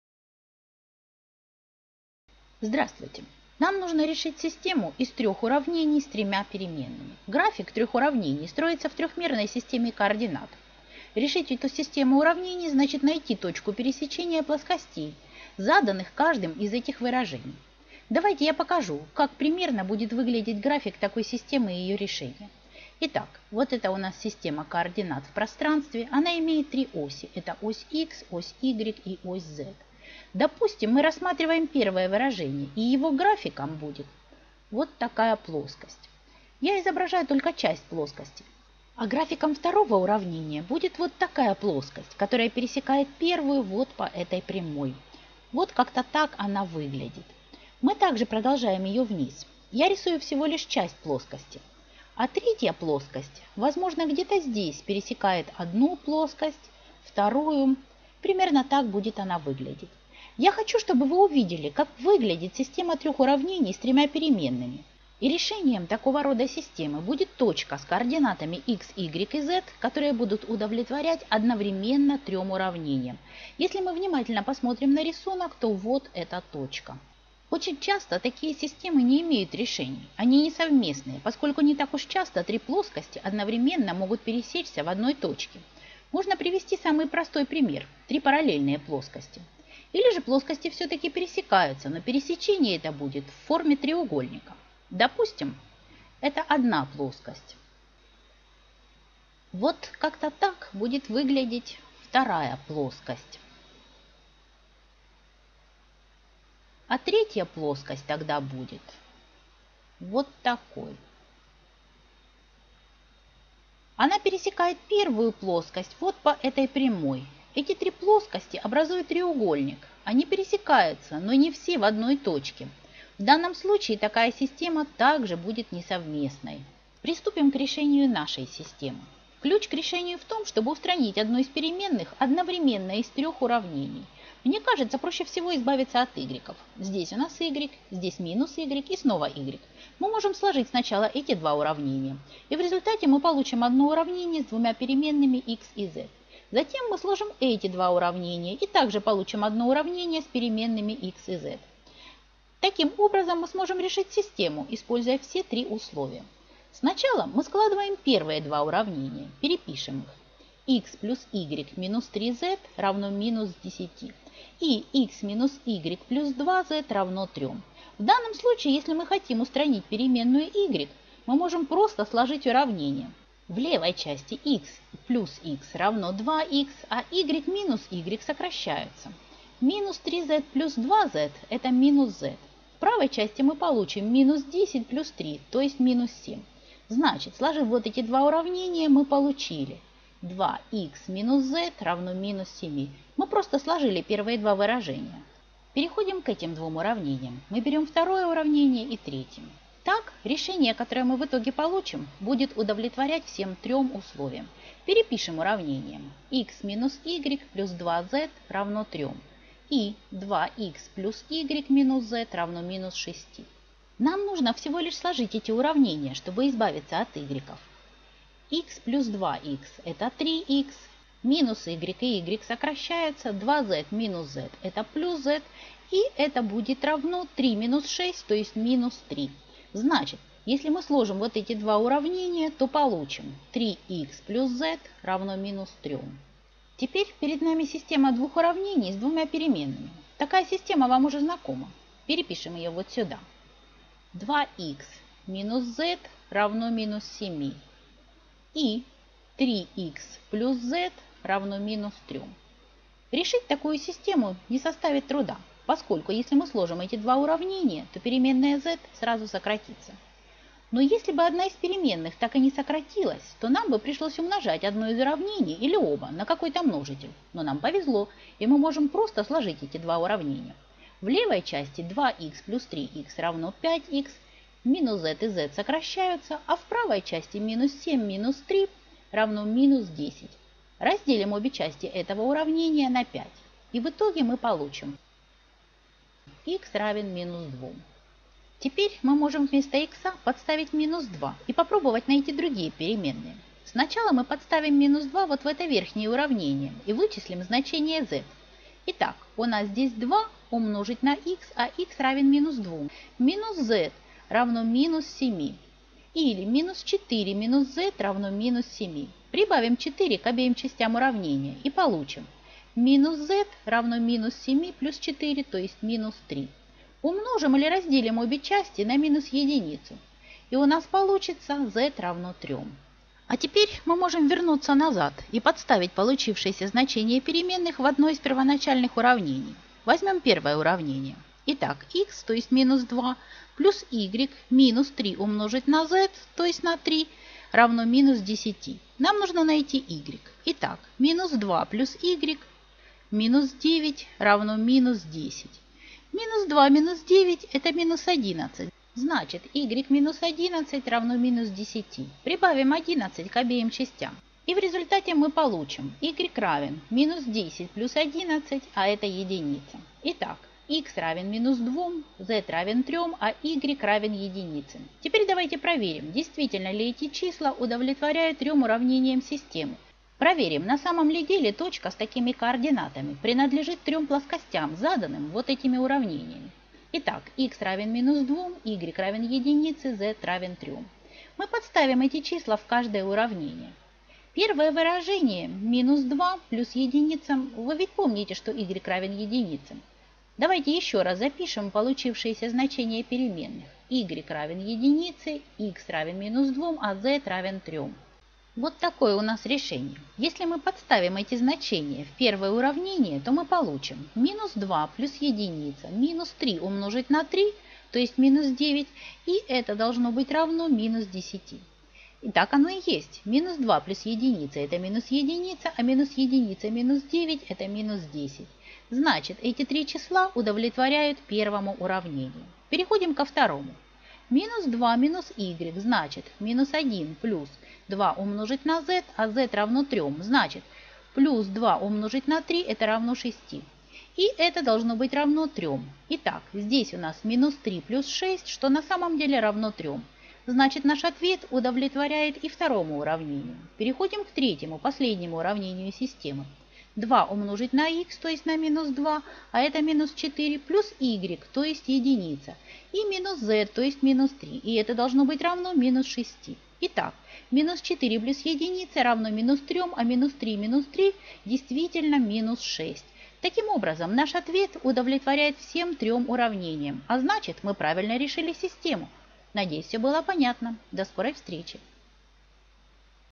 Системы уравнений с тремя переменными
В этом видео показано, как решить систему трех уравнений с тремя переменными способом сложения.

Это видео -- русская версия видео "Systems of Three Variables" Академии Хана Перевод и дублирование выполнены командой проекта «Study Planner»

This video is a Russian dubbed version of the Khan Academy video "Systems of Three Variables" The translation and sampling are made by the «Study Planner» team